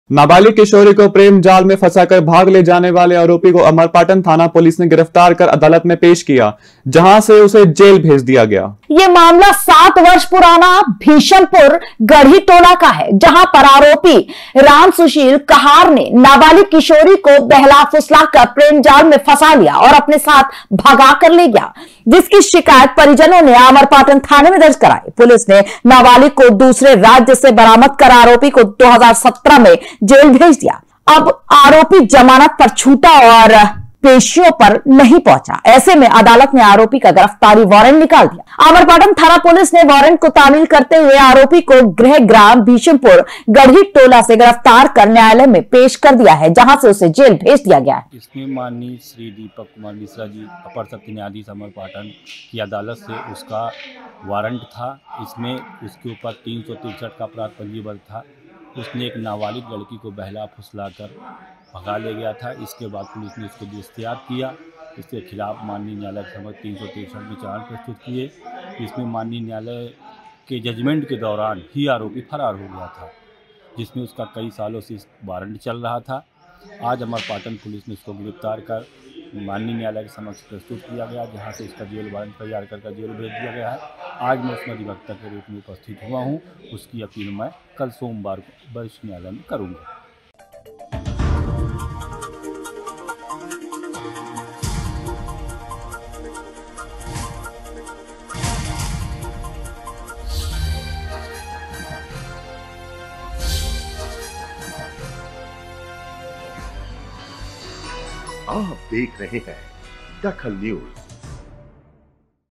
DAKHAL NEWS लड़की भगाने वाला पकड़ा गया
लड़की भगाने वाला पकड़ा गया
लड़की भगाने वाले को भेजा जेल